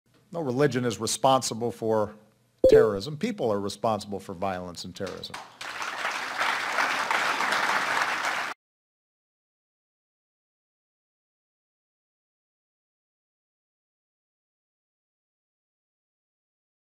Obama Says Religion don't kill people; people kill
Obama Says Religion don't kill people; people kill people. NRA Says Guns Don't kill people! People kill people! We are beginning to agree as Americans! Americans Agree!

Let the Red White and Blue Shred for you!
ShredEx Made in America!

where is your Shredding company from?
Shred-It. Canada
Recall Australia